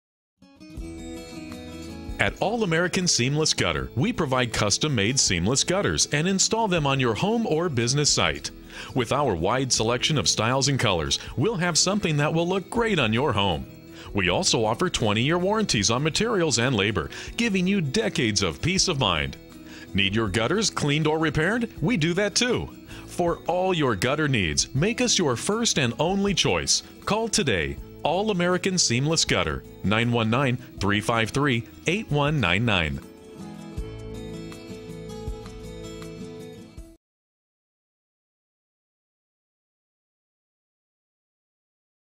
All American Aluminum Seamless Gutter Co
At All American Seamless Gutter, we provide custom made seamless gutters and install them on your home or business site. With our wide selection of styles and colors, we'll something that will look great on your home. We also offer 20 year warranties on materials and labor, giving you decades of peace of mind. Need your gutters cleaned or repaired? We do that too! For all your gutter needs, make us your first and only choice. Call today.